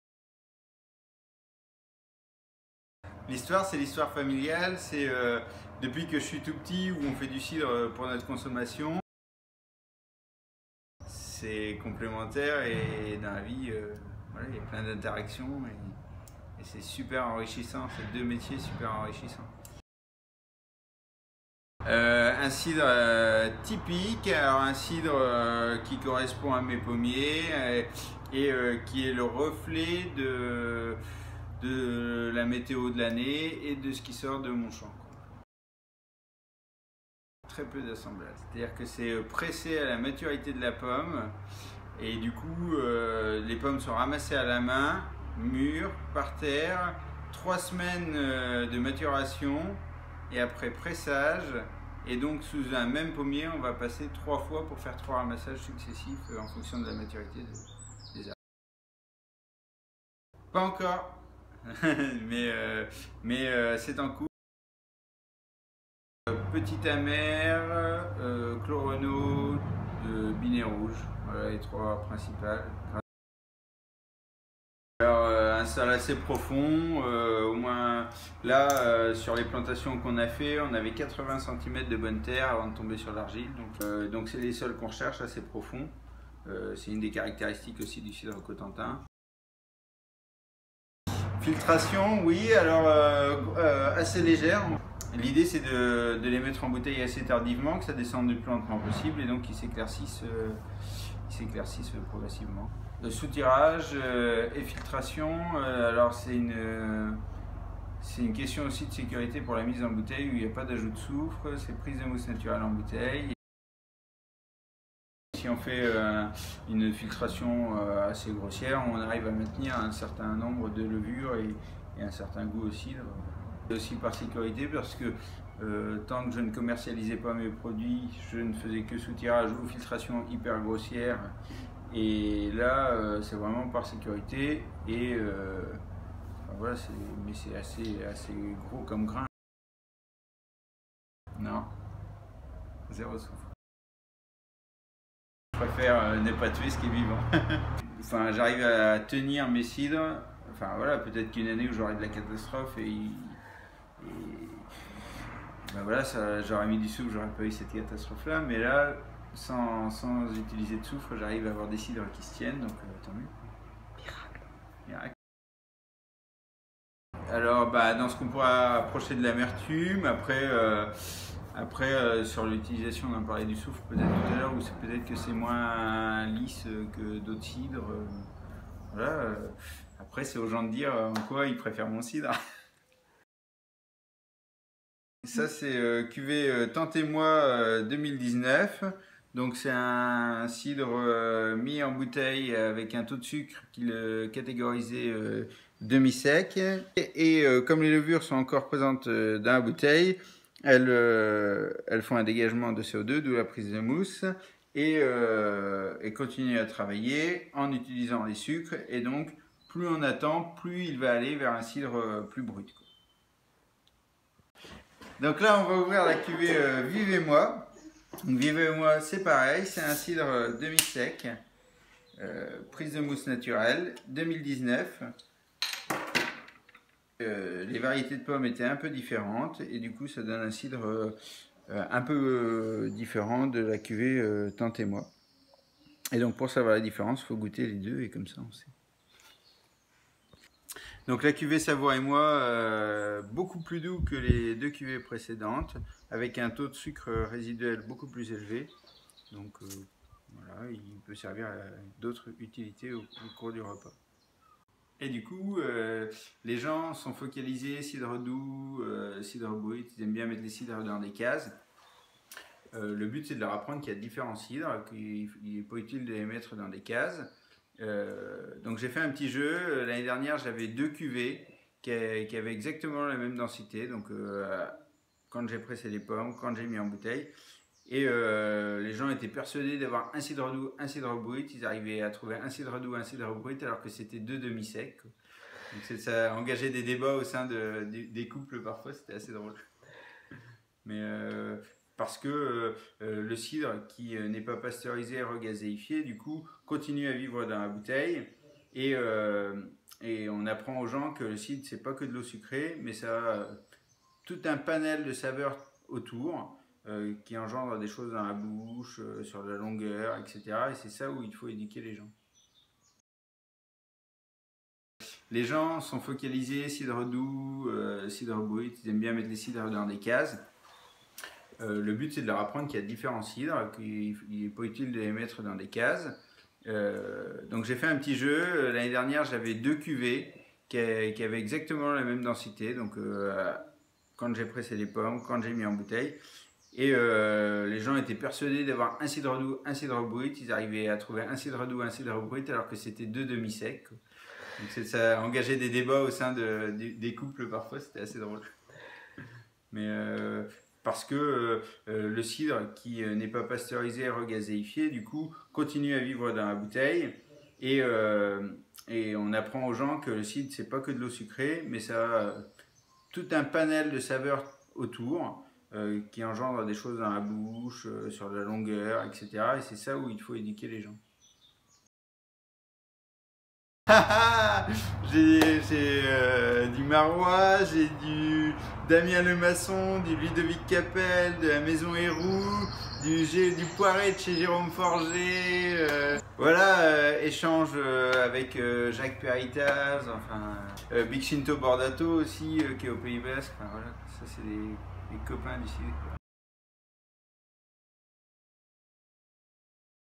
Clos Moulin CIDER - François Badier - English subtitles!
a little video presentation, where  François Badier is talking about his cider, the soil and the varieties, filtering and racking, natural fermentation, why he does not add any sulfur, why the cider should not be labeled “Demi-sec” or “Brut” and what cider does he keep at home! Instagram @applephilia